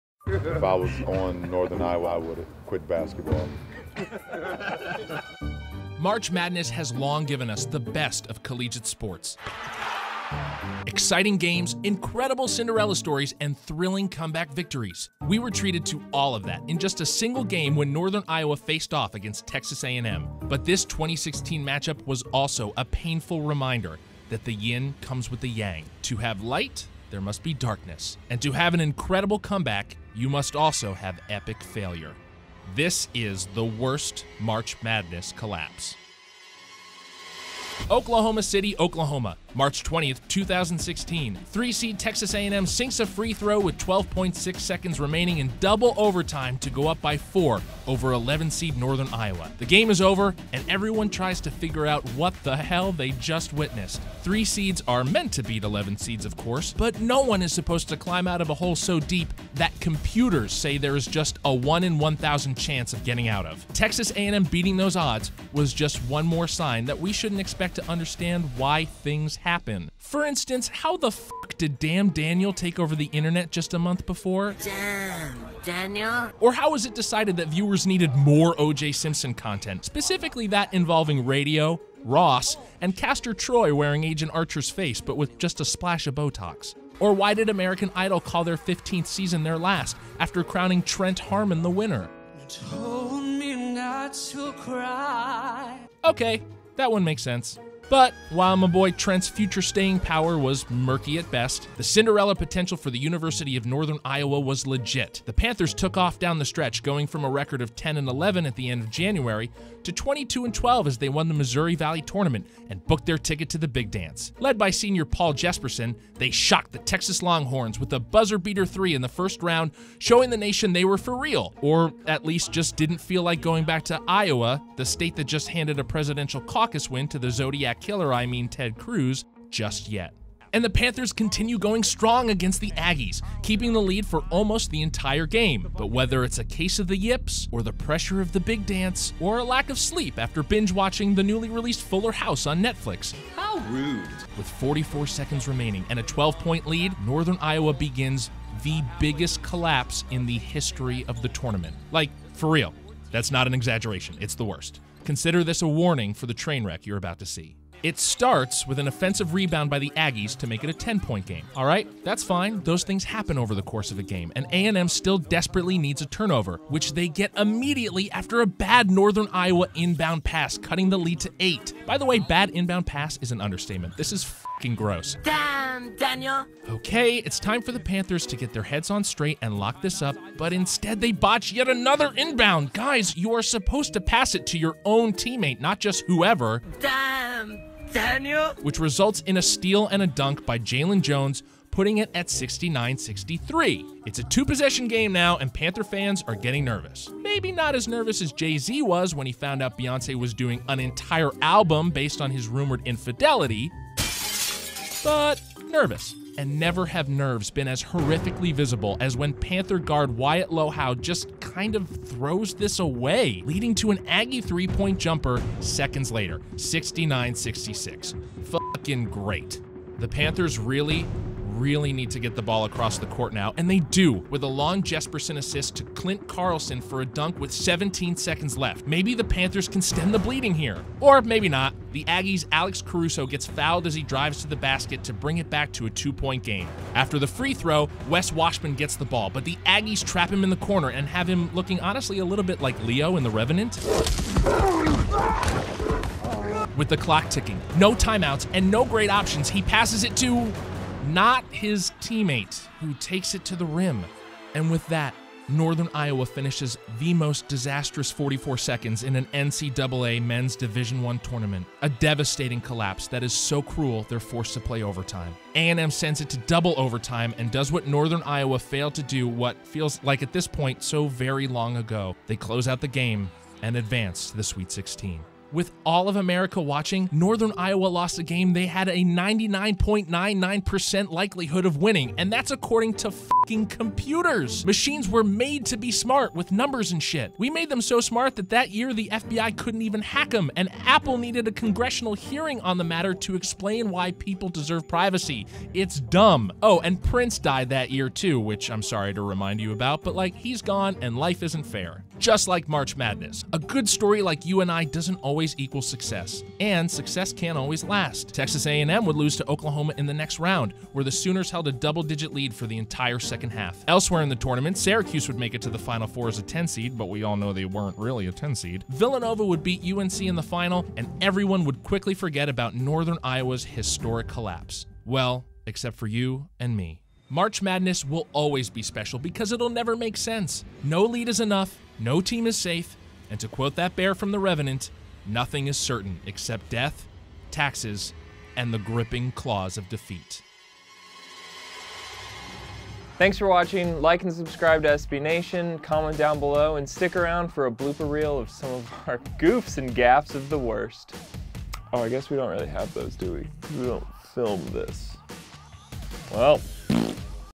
The Worst March Madness Collapse | University of Northern Iowa vs Texas A&M 2016
CORRECTION (Mar 5): The University of Northern Iowa was abbreviated as "NIU"; it should read "UNI". Also, Texas A&M was abbreviated as "Texas" which is a sin. The person responsible for these errors has been stuffed into a small box, which is a special bummer because he is quite a tall boy.

College basketball is a grind with a single champion and so many others that suffer in often gut-wrenching style. When the Cinderella darlings from Northern Iowa took on Texas A&M, the nation cheered for an upset, but all they got was a big collapse.